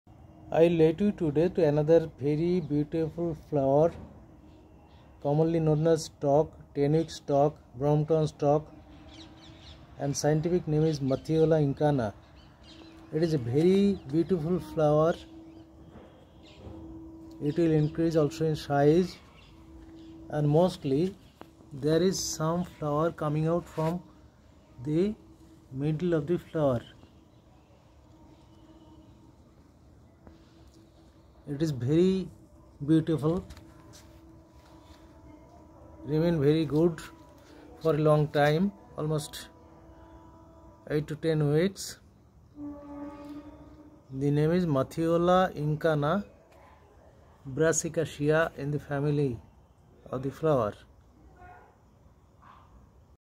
Stock flowers in my garden.
This is a very beautiful flower and long lasting also. It's name is mathiola incana. commonly called stock. It blooms in Indian climate, need little care. They prefer light and composed fertilizer.